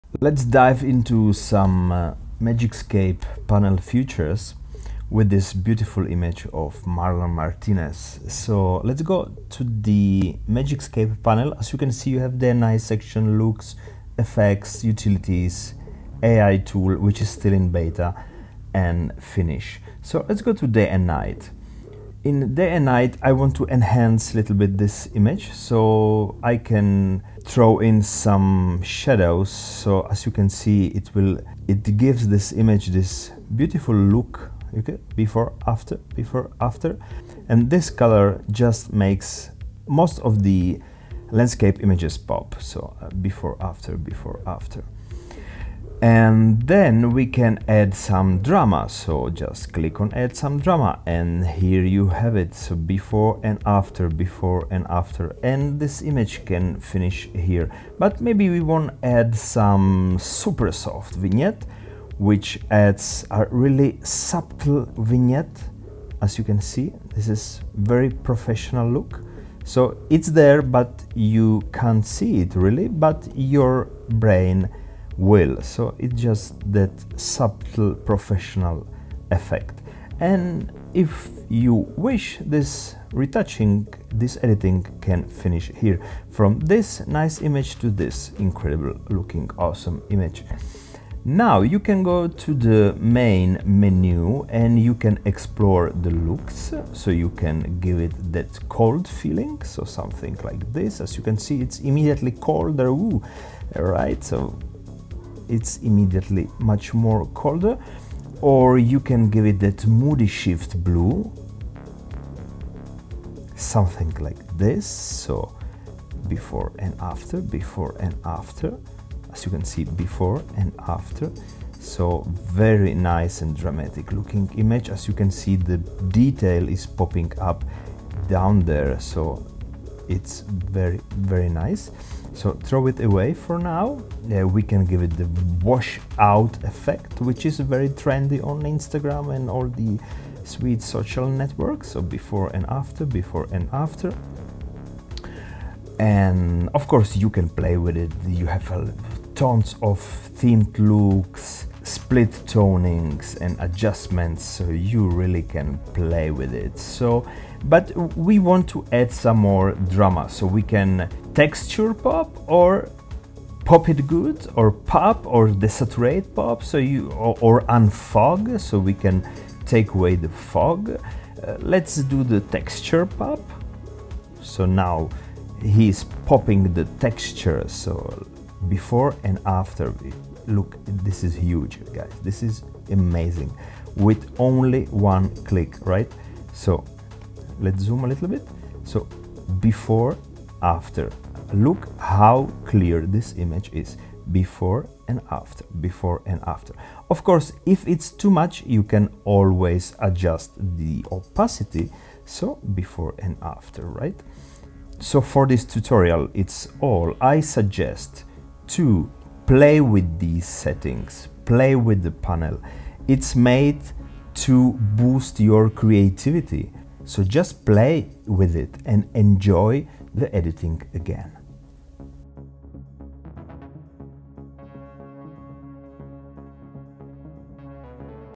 Magic Scape Panel for Photoshop Walkthrough 01
In this tutorial we will show you some of the features of the Magic Scape Panel and an editing walkthrough example. Learn how to edit landscape photos easy and fast.